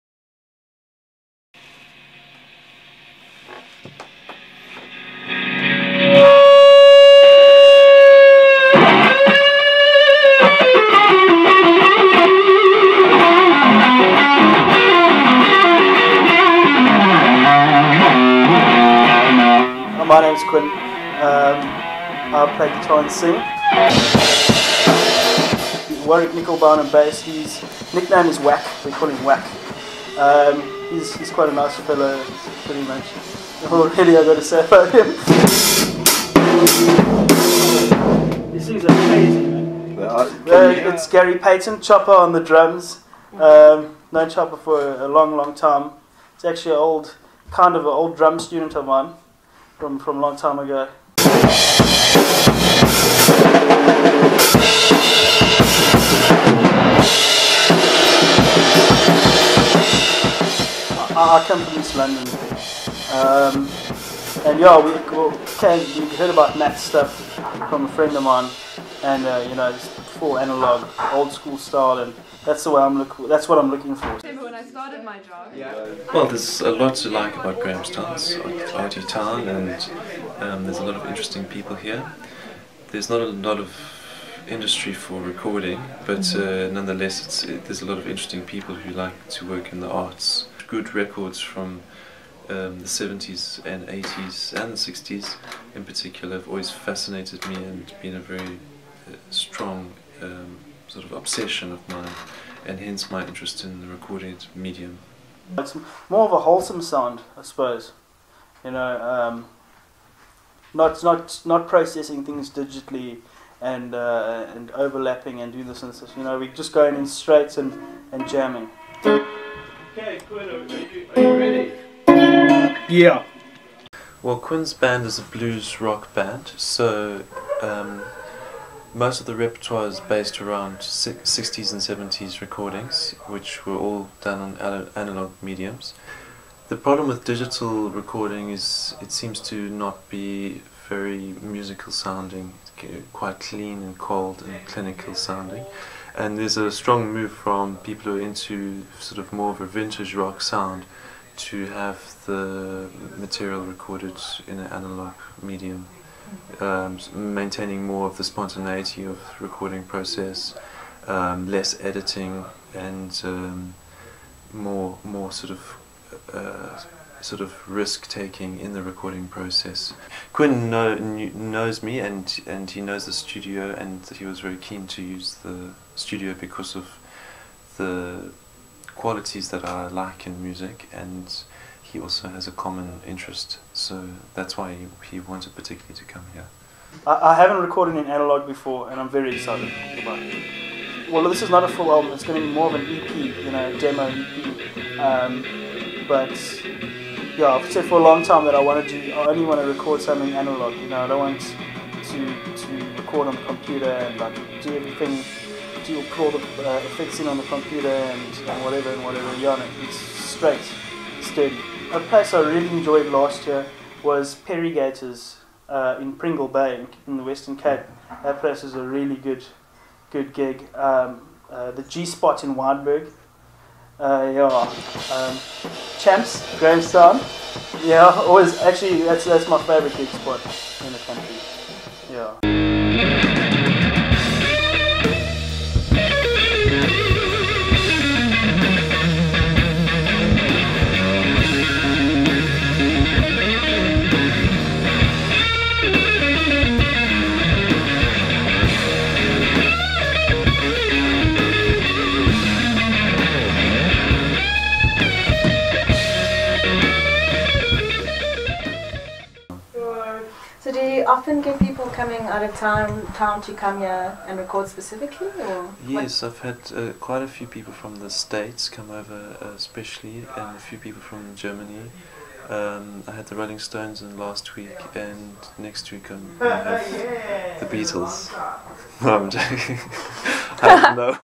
Hot act records in Grahamstown
East London-based blues rock band the Quin Maritz Trio recently recorded with sound engineer Matt Sabine in Grahamstown.

Grocott's Mail reporters spent time in studio with the group and filmed snippets of the unusual recording process they used, blending analogue and new technologies.

The trio is comprised of Quin Maritz on lead guitar and vocals, Warrick Knickelbein on bass and drummer Gary Paton.

 They said they were very excited about the process and the opportunity to record with Sabine, whose special interest is music and recording techniques from the 60s, 70s and 80s.